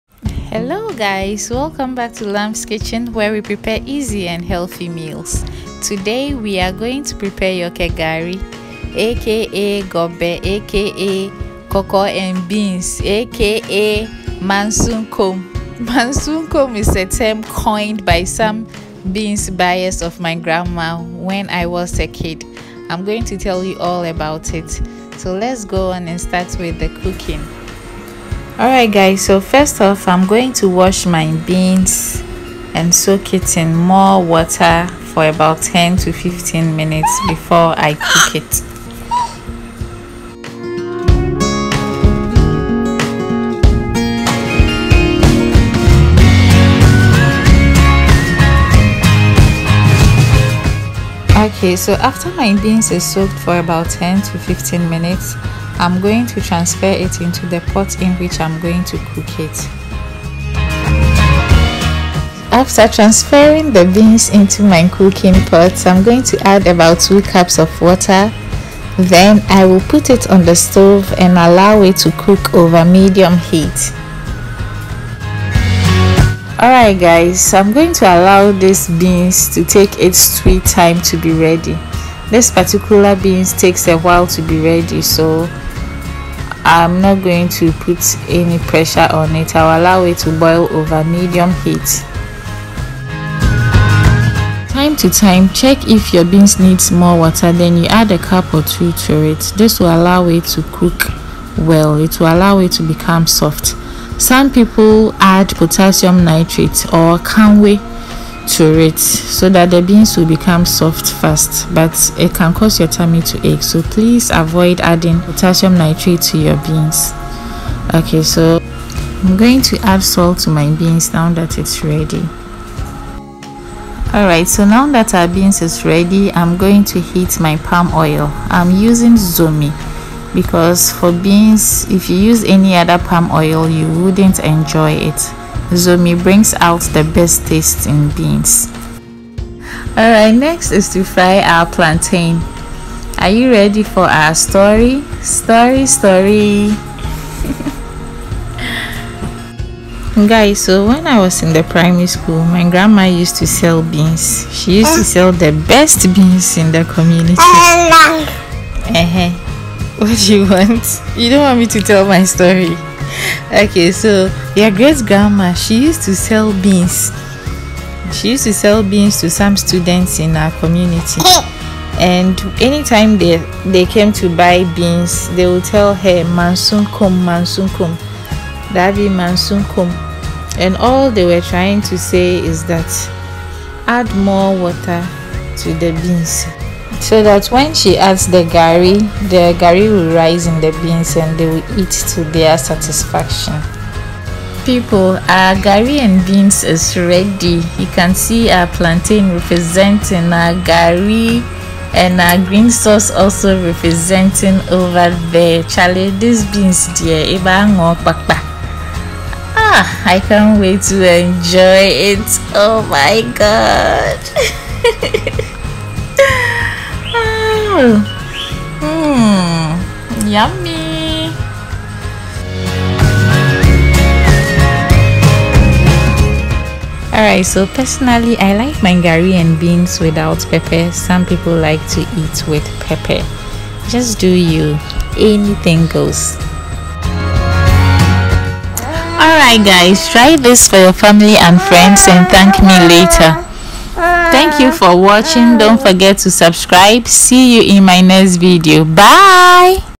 How to Make The Most Delicious Gari and Beans || Just Like Davi’s Yor ker Gari AKA Gobe
Ingredients

1. Beans
2. Plantain
3. Gari
4. Palm Oil (Zomi)
5. Vegetable oil
6. Salt

Step 1
Wash beans and soak in water for 10 to 15 mins

Step 2
Add about 2 cups of water and cook over medium heat until beans is soft. From time to time check the texture of the beans and add more water if needed

Step 3
Once beans is soft enough, add a teaspoon of salt to taste

Step 4
Slice ripe plantain and fry in heated vegetable oil until golden brown

Step 5
Heat palm oil(zomi) in a separate pot and serve on the side with your fried plantain and gari

I'm on Instagram as lamskitchengh. Install the app to follow my photos and videos.

I’m on Twitter as lamskitchengh.